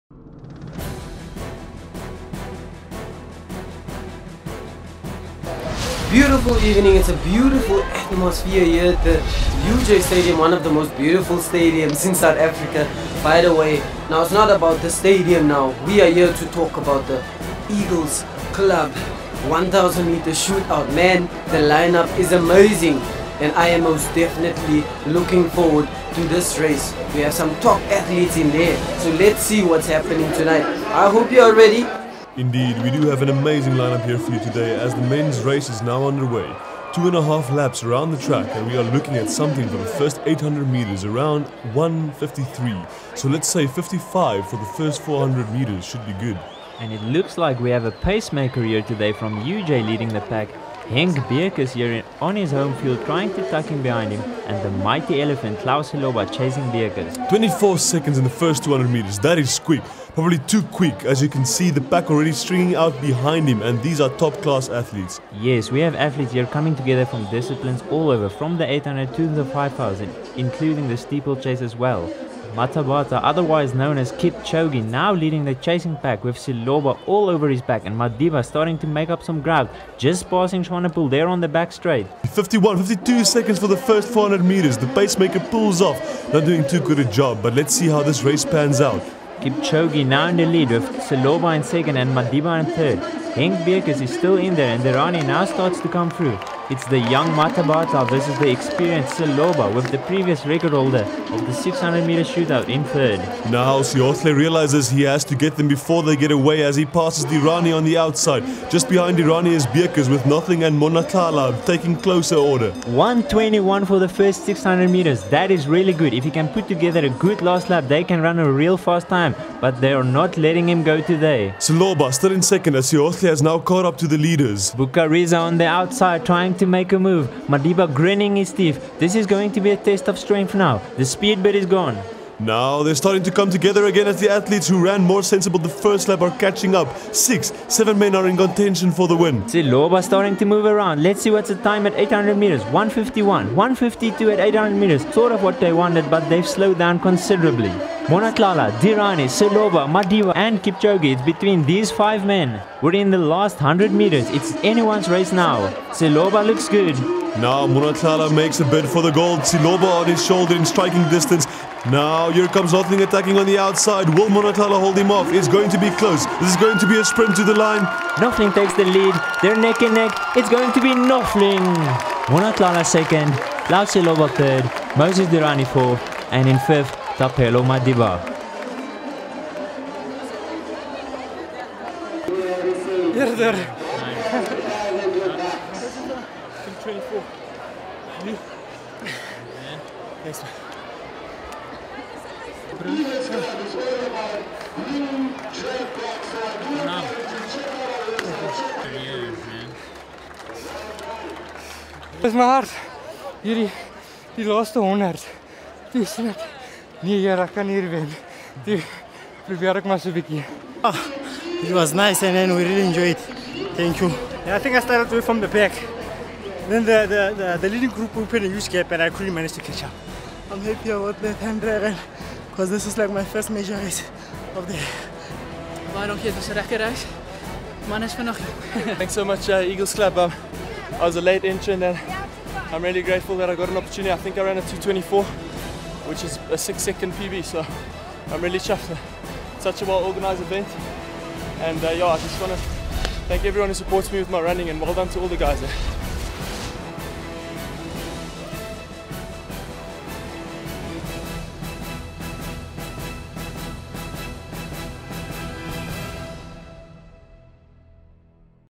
2015 Eaglesclub Classic Shootout 1000m - Men
Powered by KPMG

1 - Theodore Nothling - TUKS - 2:21.97
2 - Tumisang Monnatlala - NEDB - 2:22.44
3 - Tlou Seloba - BHAC - 2:22.81
4 - Moses Dirane - TUKS - 2:24.40
5 - Thapelo Madiba - UJ - 2:24.60
6 - Marko Bucarizza - WITS - 2:24.76
7 - Kgalagelo Mathanatha - SOWKEN - 2:25.47
8 - Clinton Swanepoel - UJ - 2:26.13
9 - Tumelo Motlagale - UJ - 2:28.32
10 - Folavio Sehohle - NEDB - 2:28.84
11 - Henk Beukes - UJ - 2:30.35
12 - Bruce-lynn Damons - EC - 2:32.23